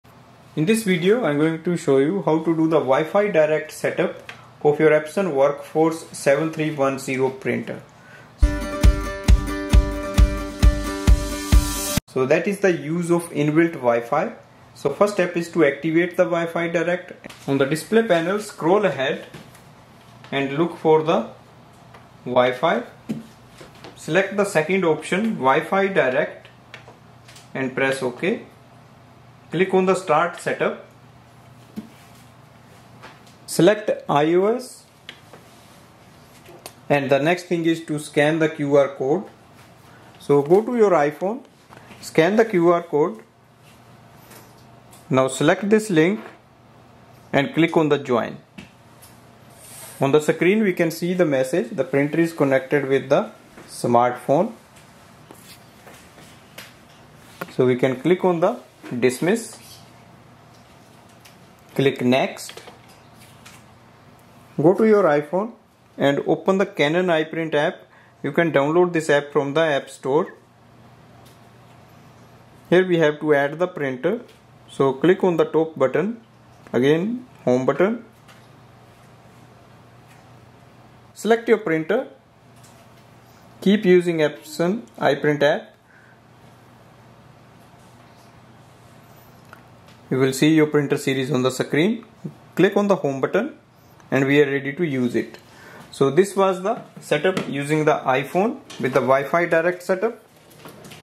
Epson WorkFore WF-7310 WiFi Direct Setup !
This video reviews the method to do the wireless WiFi Direct Setup of Epson Workforce WF-7310 Printer using the control panel and iPhone or Android Phone. This shows how to connect to the smartphone using the printer network.